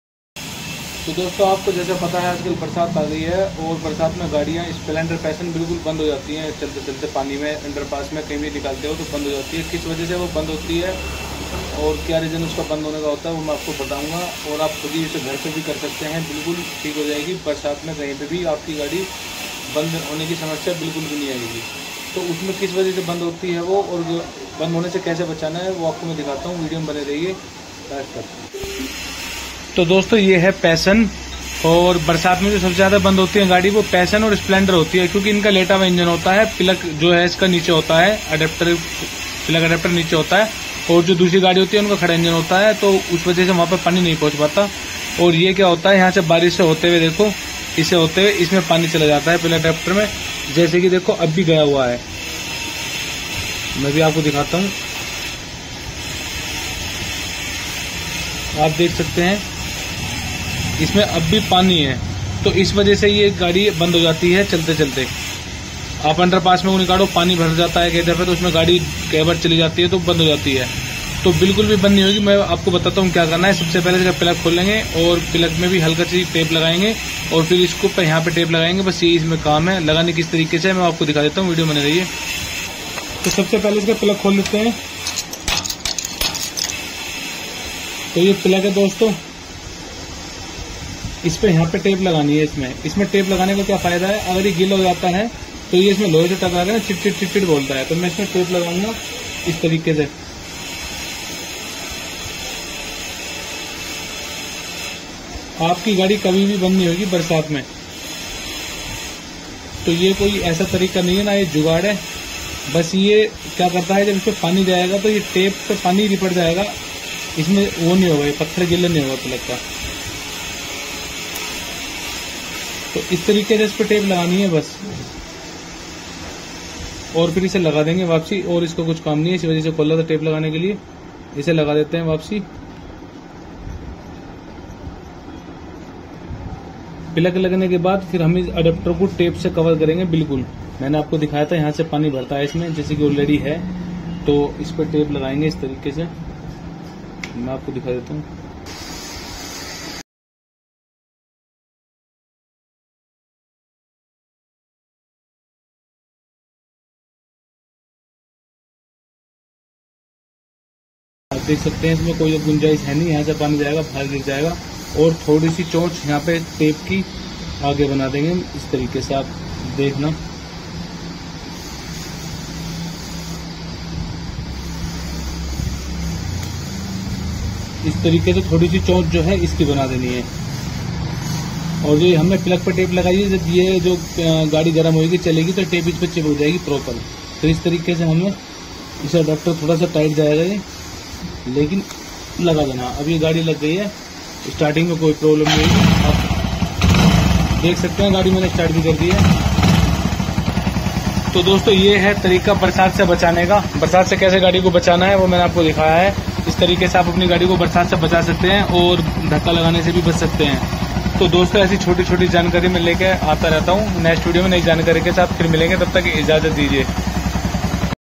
अब नहीं होगी बरसात में गाड़ी बंद Hero Passion Pro
Barsat Mein Kyon Ho Jaati Hai gadi band nice video mein bataya hai Barsat Mein gadi band hone se Kaise bachayen aur uske liye Kya Karen  Hero Passion Pro  Aman bike point